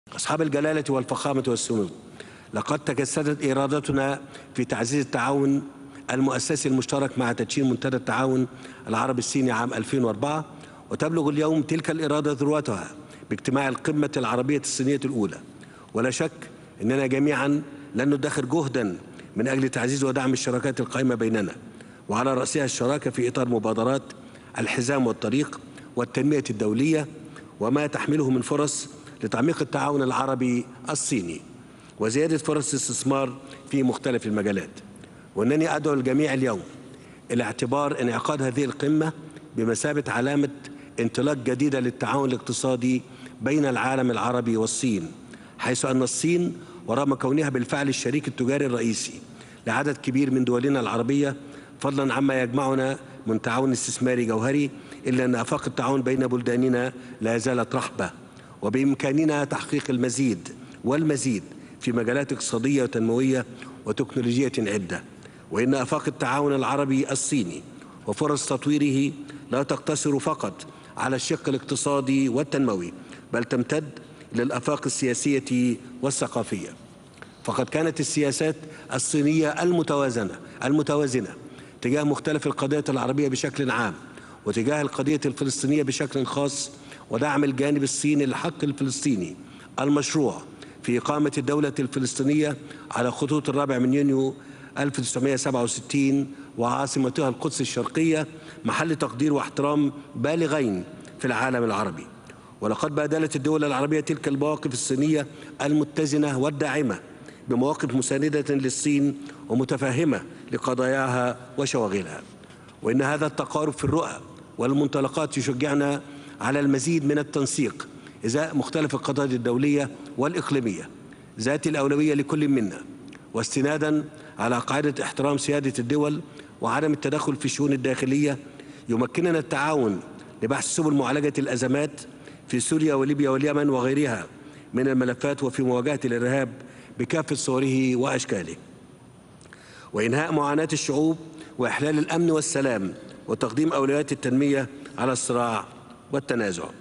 الرئيس السيسي: أدعو الجميع لاعتبار هذه القمة انطلاقة لتعزيز التعاون بين الدول العربية والصين
تغطية خاصة | الرئيس السيسي: أدعو الجميع لاعتبار هذه القمة انطلاقة لتعزيز التعاون بين الدول العربية والصين